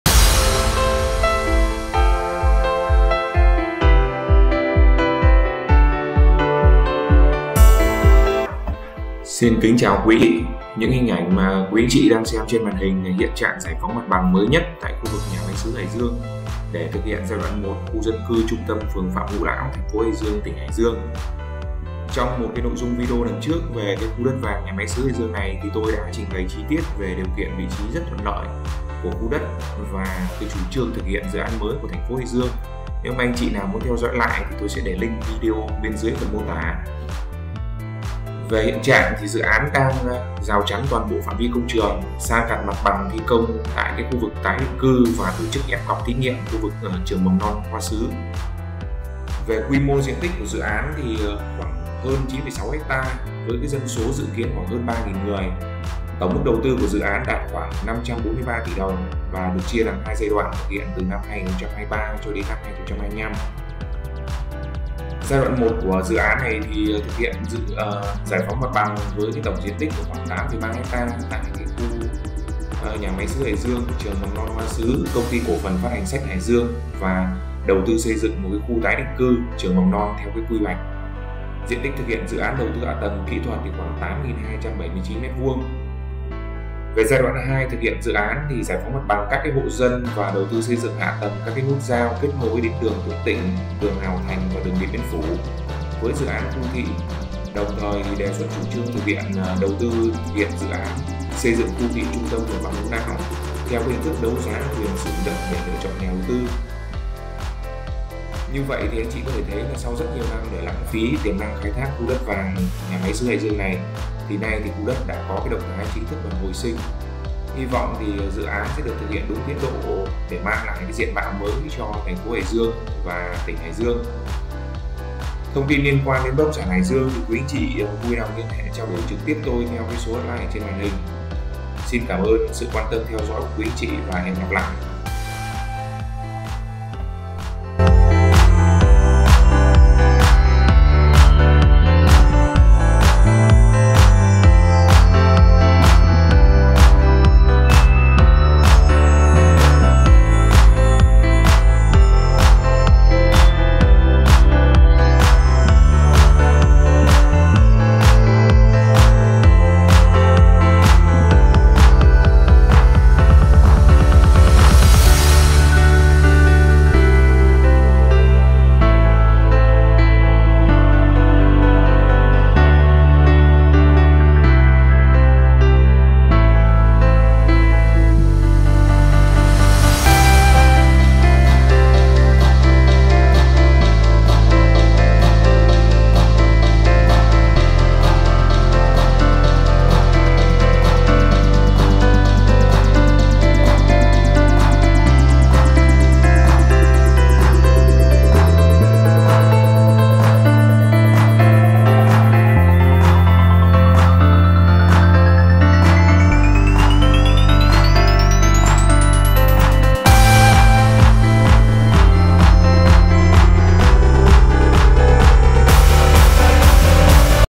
Hiện trạng giải phóng mặt bằng Nhà Máy Sứ Hải Dương GĐ 1 tháng 10/2023. Hồi sinh khu đất vàng?
Những hình ảnh anh/chị đang xem là hiện trạng giải phóng mặt bằng mới nhất tại khu vực nhà Máy Sứ Hải Dương đê thực hiện giai đoạn 1 khu dân cư trung tâm phường Phạm Ngũ LÃo, tp. Hải Dương, tỉnh Hải Dương.

Trong nội dung video lần trước về khu đất vàng nhà máy Sứ Hải Dương tôi đã trình bày chi tiết về điều kiện vị tri rất thuận lợi của khu đất này và chủ trương thực hiện dự án mới của Tp. Hải Dương. Anh/chị nào muốn theo dõi lại tôi sẽ để link video bên dưới phần mô tả.

Về hiện trạng thì dự án đang rào chắn toàn bộ phạm vi công trường, san gạt mặt bằng thi công khu tái định cư và tổ chức ép cọc thí nghiệm khu vực Trường Mầm non Hoa Sứ.

Quy mô diện tích dự án hơn 9,6 ha, dân số dự kiến khoảng 3.000 người. Tổng mức đầu tư khoảng 543 tỷ đồng. được chia làm 2 giai đoạn thực hiện từ năm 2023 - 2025

Giai đoạn 1 thực hiện dự án giải phóng mặt bằng (GPMB) với tổng diện tích hơn 8,3 ha tại khu Nhà máy Sứ Hải Dương, Trường Mầm non Hoa Sứ, Công ty Cổ phần Phát hành sách Hải Dương và đầu tư xây dựng khu tái định cư, trường mầm non theo quy hoạch. Diện tích thực hiện dự án đầu tư hạ tầng kỹ thuật khoảng 8.279 m2.

Giai đoạn 2 thực hiện dự án GPMB các hộ dân và đầu tư xây dựng hạ tầng các nút giao kết nối đường Tuệ Tĩnh, đường Hào Thành và đường Điện Biên Phủ với dự án khu đô thị. Đồng thời đề xuất chủ trương đầu tư thực hiện dự án đầu tư xây dựng khu đô thị trung tâm phường Phạm Ngũ Lão (khu vực 1) theo hình thức đầu giá quyền sử dụng đất để lựa chọn nhà đầu tư.
Như vậy, có thể thấy sau rất nhiều năm để lãng phí tiềm năng khai thác khu đất nhà máy sứ Hải Dương thì nay khu đất đã có động thái chính thức hồi sinh. Hi vọng dự án được thực hiện đúng tiến độ để mang lại diện mạo mới cho Tp.

Xin cảm ơn sự quan tâm theo dõi của quý anh/chị và hẹn gặp lại!